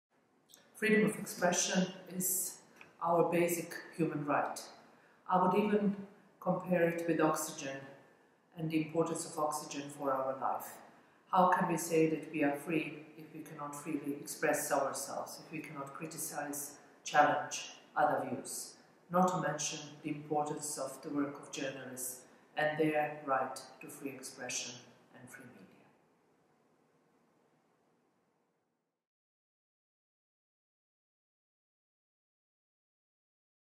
MyFreeExpression Dunja Mijatovic
MyFreeExpression is akin to oxygen. We need it to survive, says OSCE​'s Dunja Mijatović. What does FreedomOfExpression mean to you?